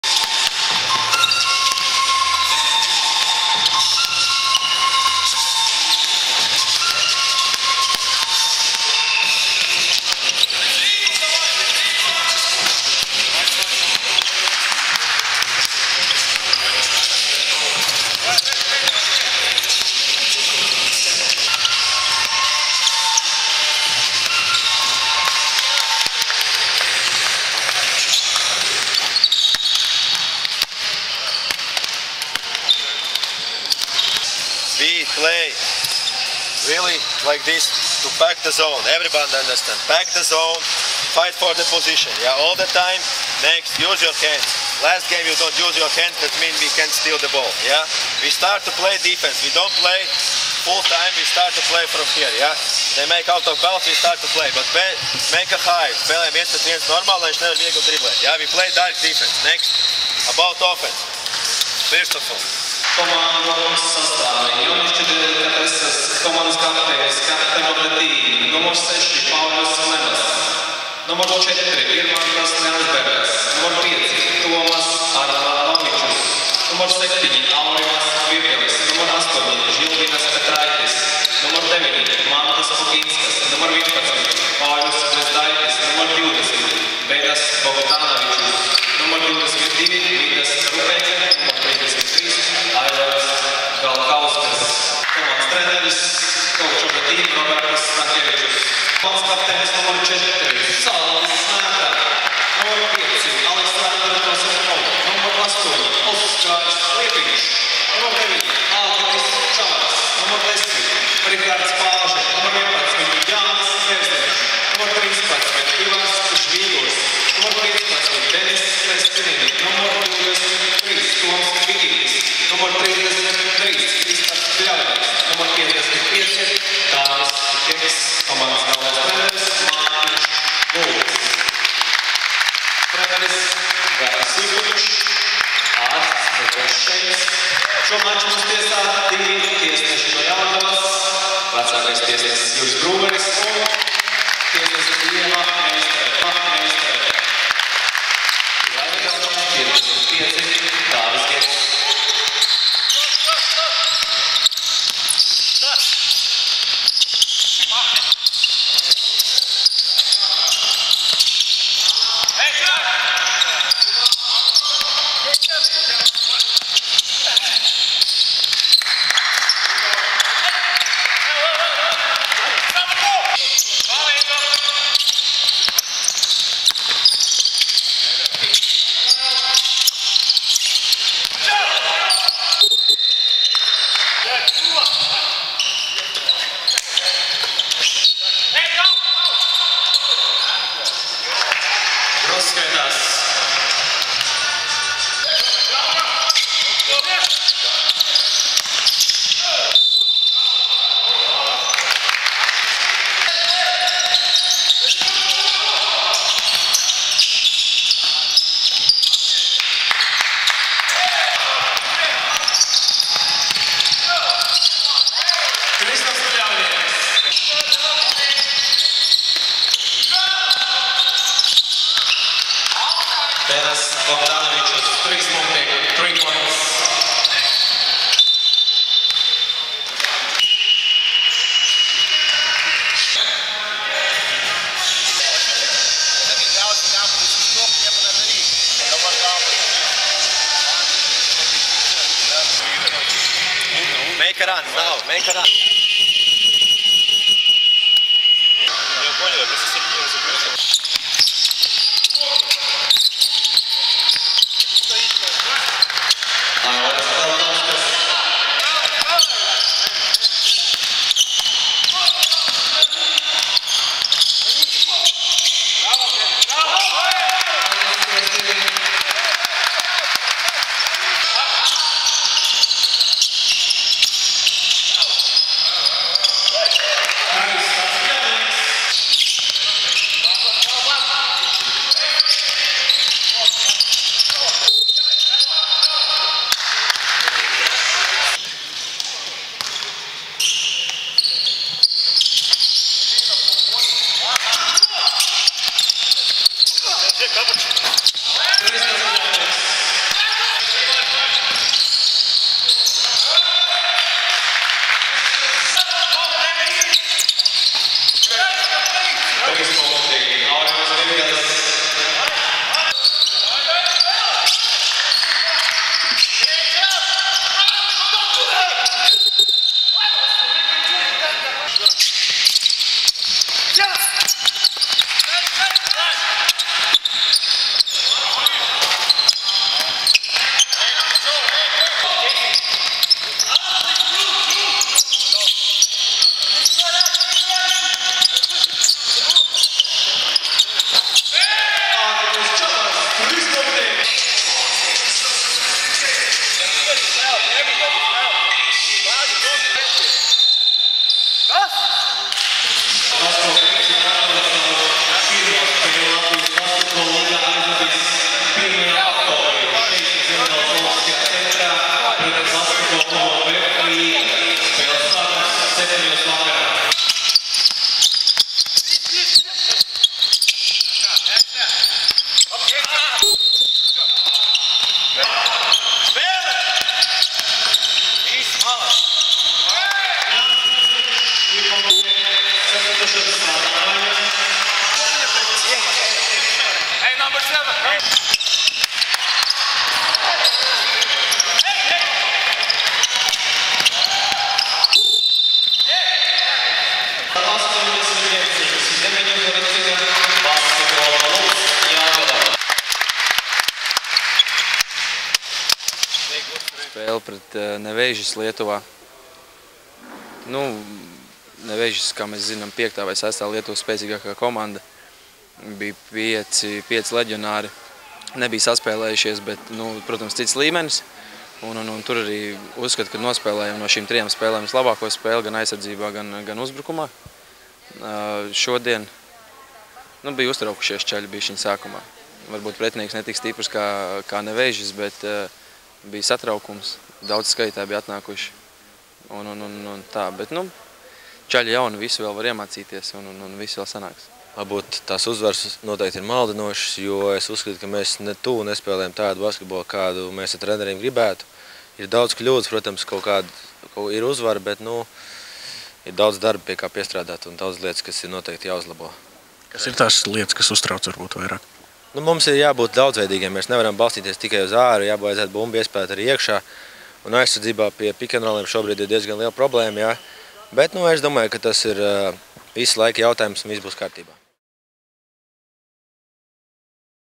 140920 BKJelgava_DelikatesesJoniški_PārbSpēleJelgavā_8min
2014.g.20.septembris.Basketbols.Pārbaudes spēle Jelgavā.
BK Jelgava - BK Delikateses Jonišķi.
Pēcspēles intervijas: Mārtiņš Gulbis, Gatis Justovičs.
Video: Māris Martinsons, Krišs Upenieks.